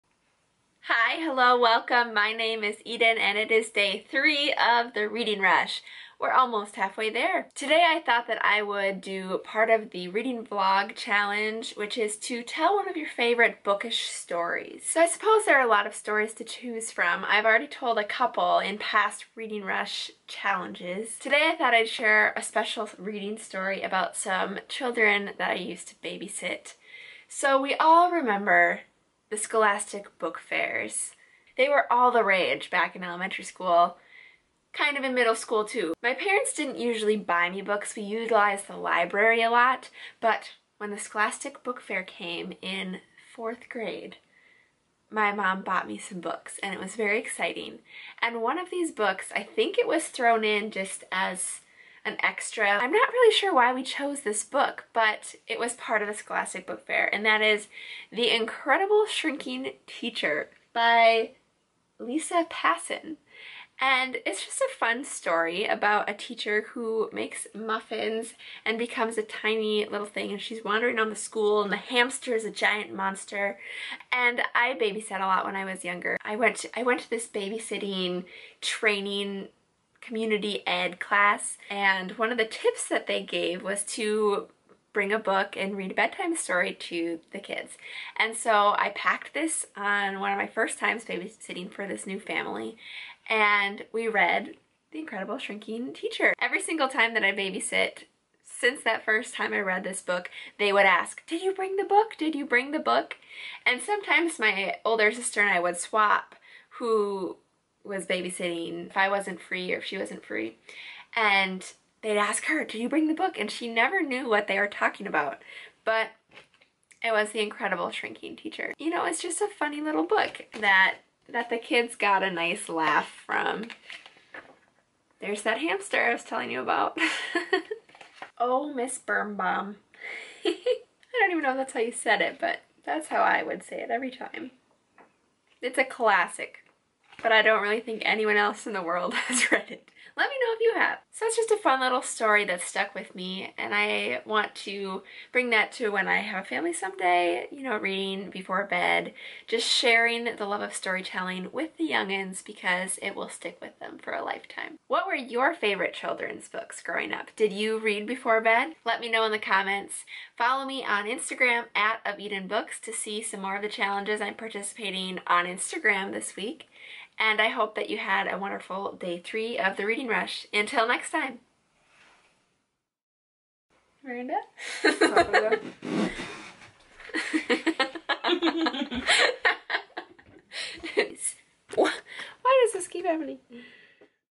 Reading Rush Day 3 || 2020
STORY TIME! I tell one of my favorite bookish stories.
Did you have favorite children's books growing up?